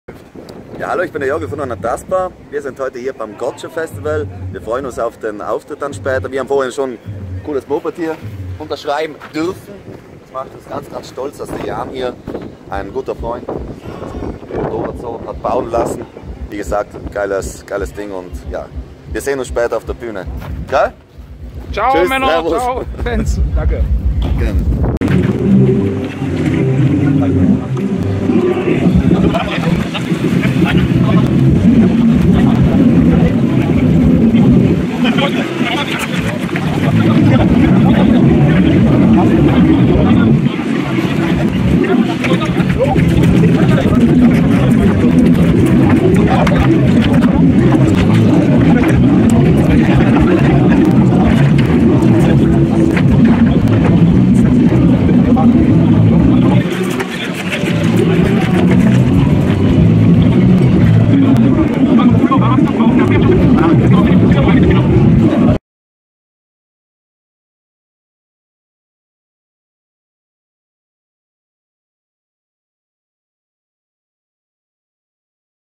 Joggl Making off UNANTASTBAR von Vomue
Hier noch einmal eine mitlaufende Kamera beim Goitschefestival. Die Band UNANTASTBAR signiert das Fanbike von Jan . Einer Ihrer größten Fans. Goitschefestival 2017